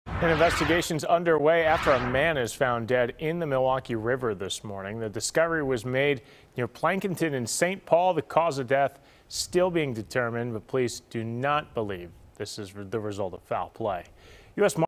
Body found in Milwaukee River | FOX6 News Milwaukee
The Milwaukee Fire Department's Dive Response Team were dispatched to the scene near Plankinton and St. Paul Wednesday morning.